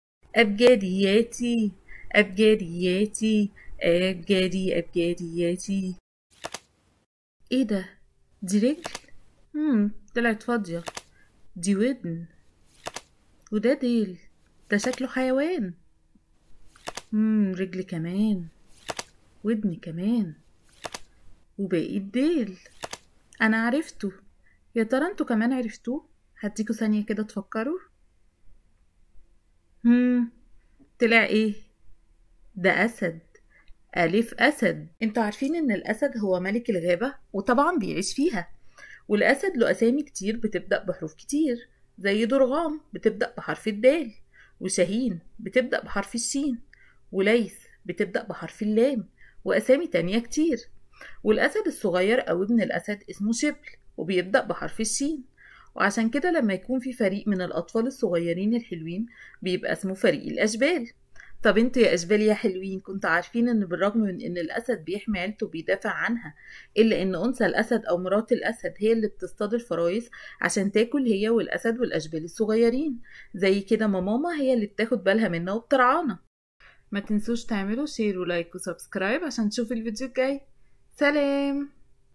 أبجدياتي | ألعب و اتعلم حروف الأبجديه العربيه للأطفال | حرف الألف اسد | بدون ايقاع
أتعلم كلمه جديده بتبدأ بحرف الألف و معلومات كتير عن الكلمه دي. بدون موسيقي و بدون ايقاع
 كلمه النهارده هي اسد. اتعلم معلومات و اسامي الاسد  المختلفه

العب و اتعلم حروف الهجاء هي سلسله لتعليم الحروف العربيه و في نفس الوقت معلومات عن مملكه الحيوانات